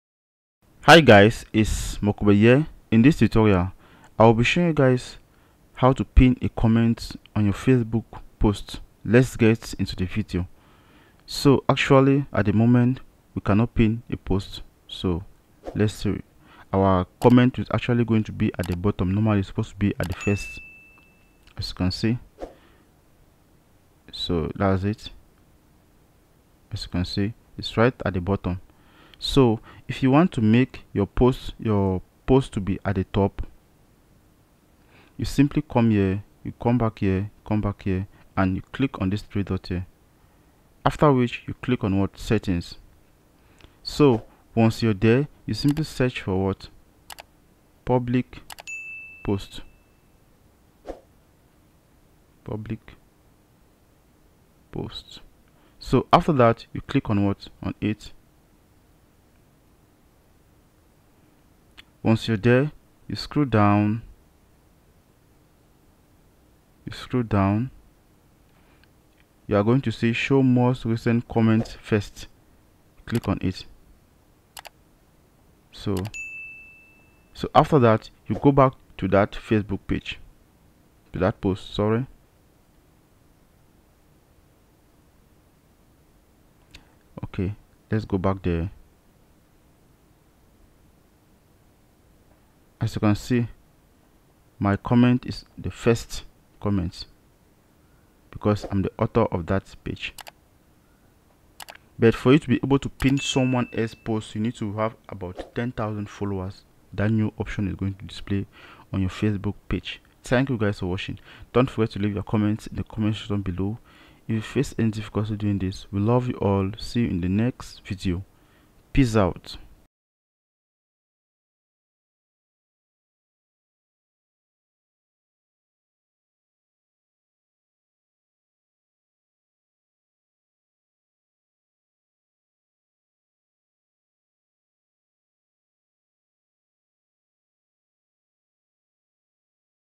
How to Pin a Comment in Facebook Post (Easy Method)
If you're wanting to Pin a comment in a Facebook post, then this video is for you! In this video, we'll show you how to Pin a comment in a Facebook post so that it stays pinned in the post, even if you leave the page and come back later.

Being able to Pin a comment in a Facebook post is a great way to save it for later. Pinning a comment will keep it visible in the post, even if you leave the page and come back later. This is a great way to keep track of comments you want to answer later, or to keep a comment pinned near the top of a post for easy access. Watch this video to learn how to Pin a comment in a Facebook post